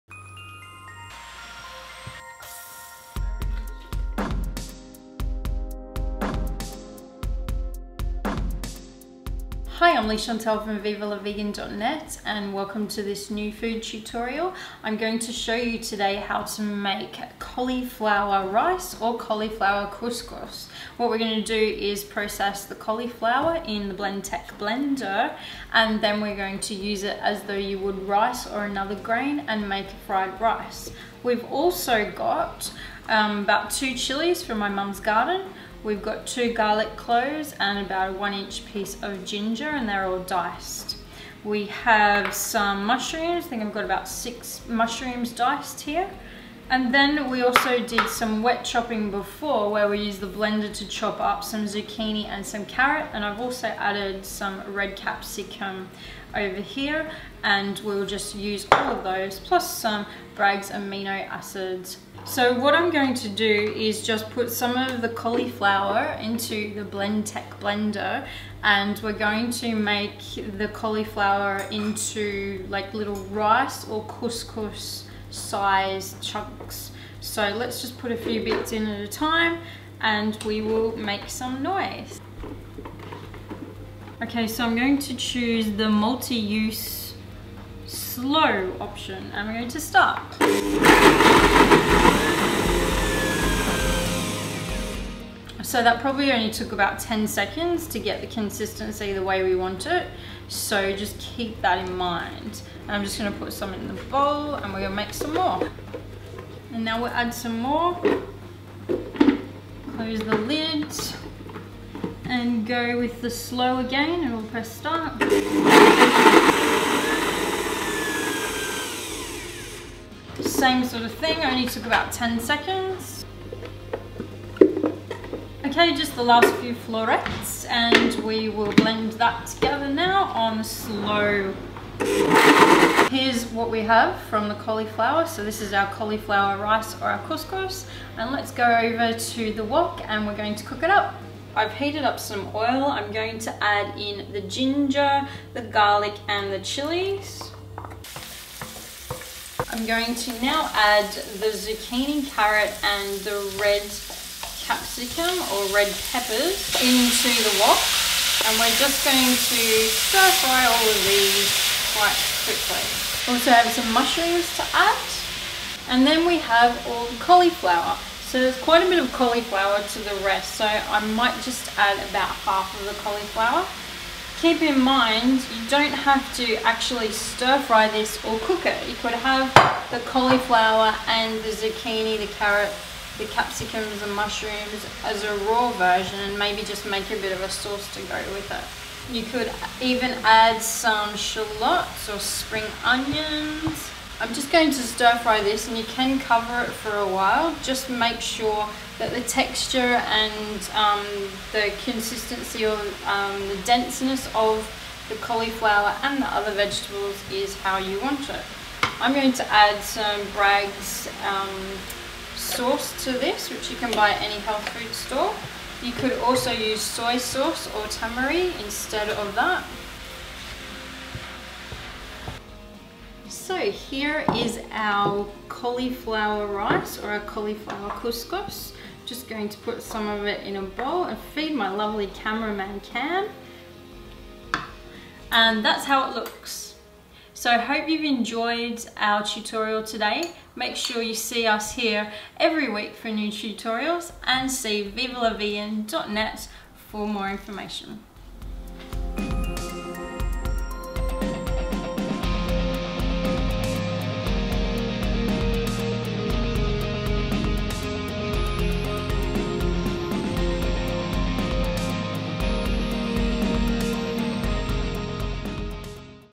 How to Make Vegan Cauliflower Rice / Cauliflower Couscous
Leigh-Chantelle from Viva la Vegan! shows you how to make Cauliflower Rice / Cauliflower Couscous.